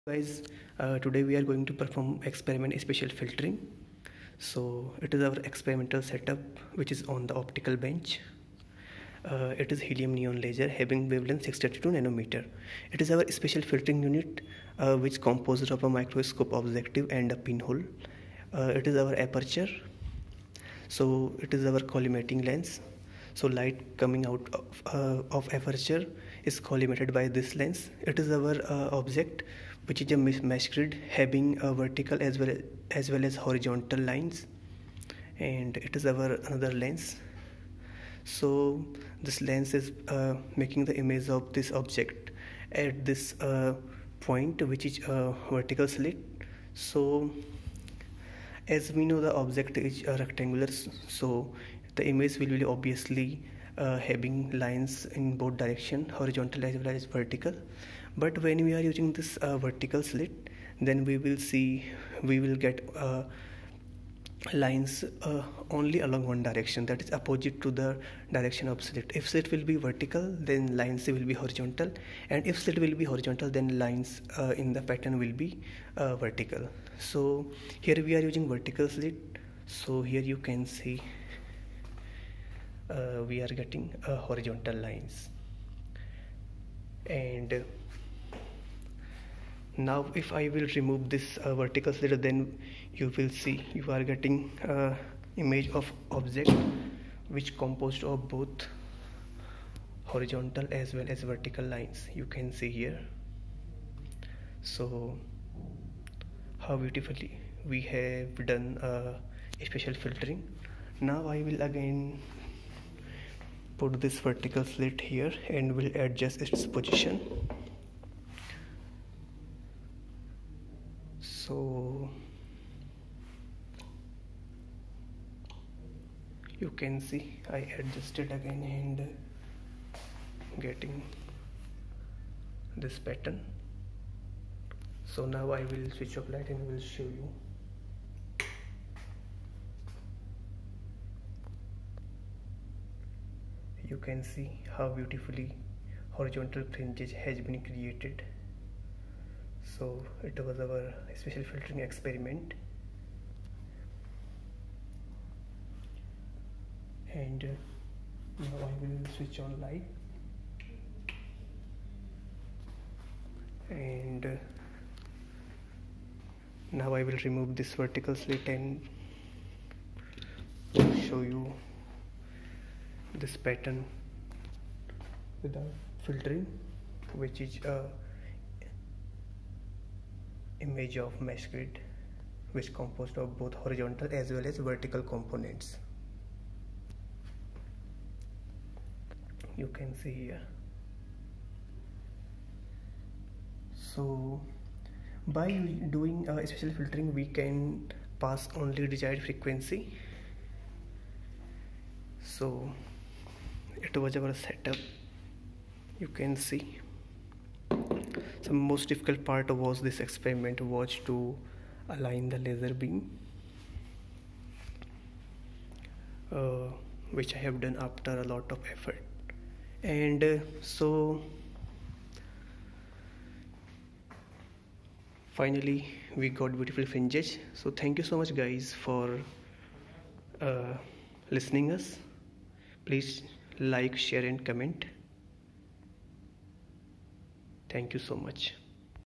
Spatial Filtering Experiment (Optics Experiment)
The main motive behind this experiment is to filter out desired spatial frequency .  In addition of spatial filtering unit which is a microscope objective and pin hole we can also use slits as a filter which allow only frequencies along one spatial axis.